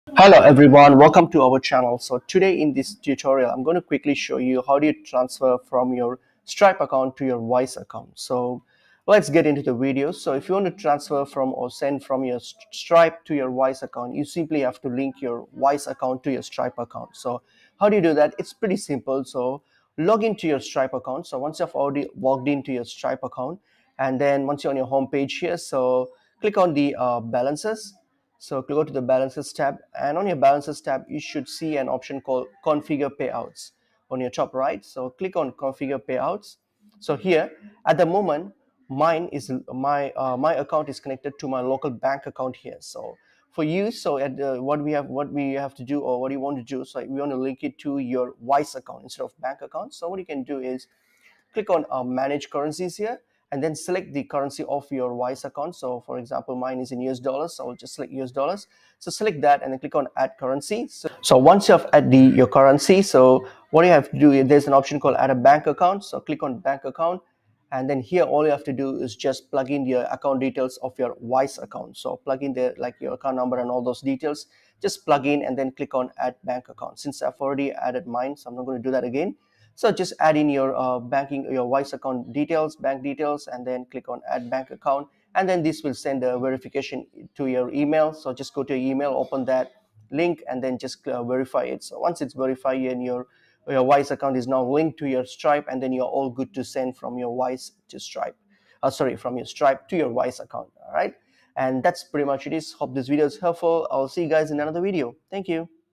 How to Send From Stripe to Wise (EASY)
Are you trying to send something from your stripe account to your wise account, but you're not sure how to do it? In this video, we'll show you how to send something from stripe to wise.

This is a common question that people ask and we're here to help! In this video, we'll show you how to send something from stripe to wise step by step. Whether you're sending a text message, an email, or a file, we've got you covered! So watch this video and learn how to send from stripe to wise!